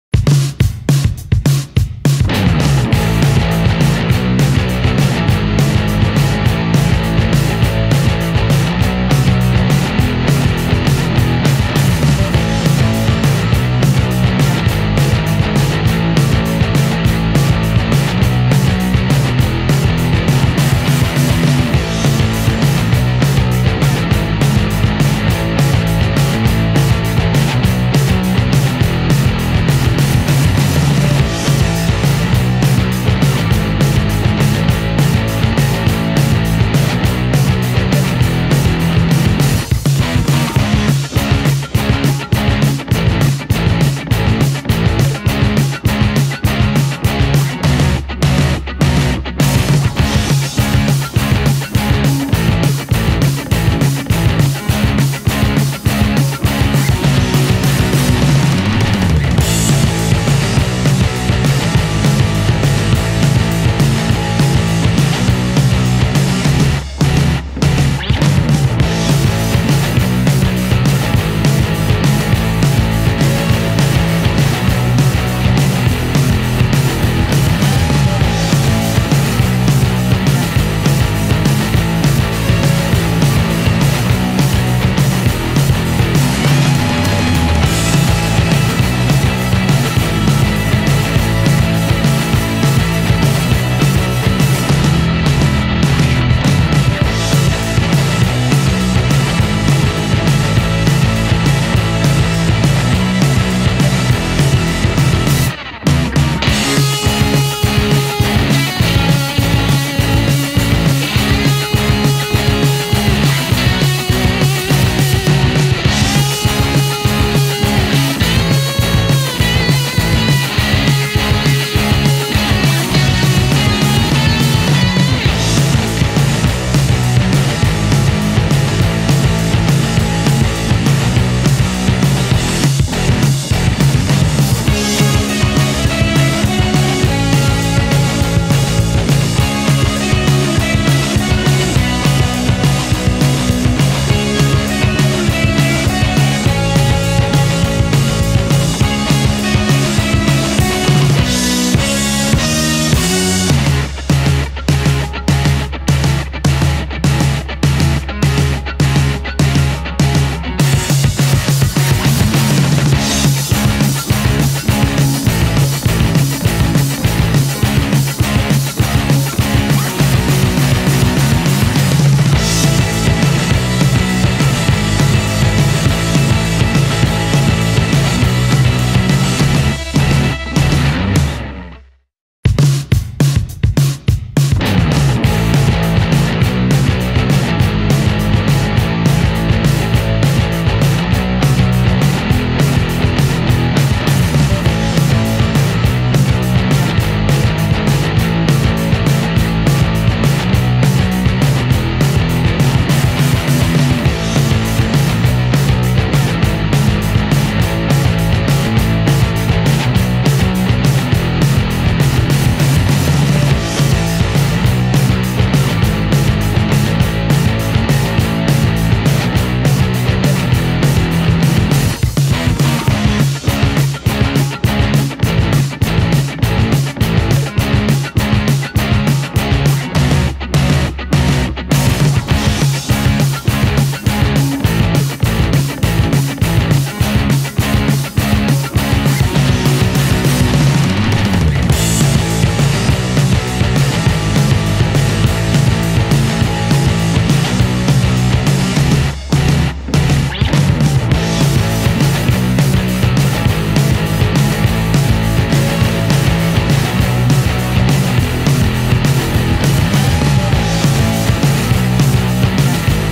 Drawing Ace, Luffy and Sabo
Drawing Luffy and his two brothers Ace and Sabo from One Piece

I used a reference from @SergiART on Deviantart ✍️